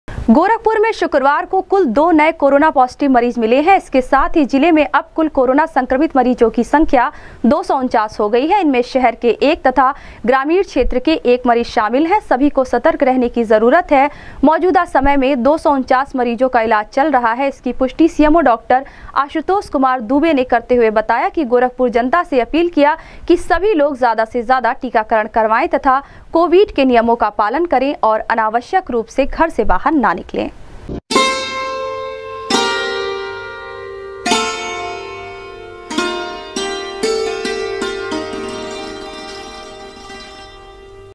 गोरखपुर में शुक्रवार को पाए गए 2 नए कोरोना संक्रमित मरीज, जिले में अब एक्टिव केस की संख्या
गोरखपुर में  शुक्रवार को पाए गए 2 नए कोरोना संक्रमित मरीज, जिले में अब एक्टिव केस की संख्या हुई 249 ,सीएमओ डॉक्टर आशुतोष कुमार दुबे ने की पुष्टि ।।।